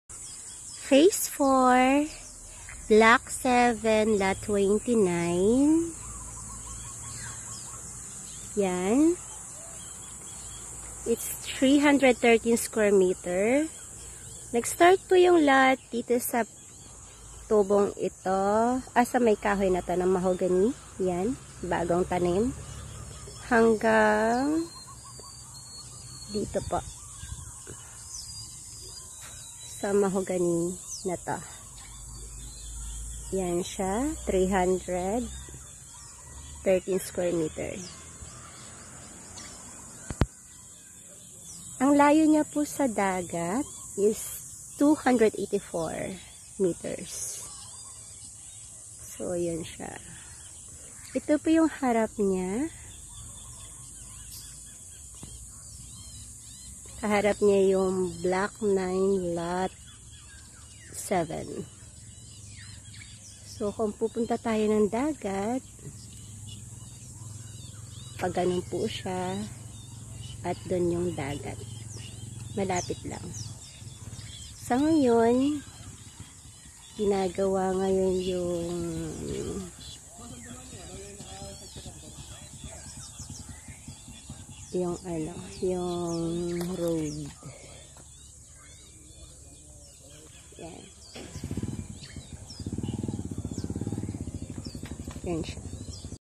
Playa Laiya Residential Phase 4 Block 7 Lot 29
Lot size: 313sqm.
Walking distance to beach and Club House.

Ms. Riza Caling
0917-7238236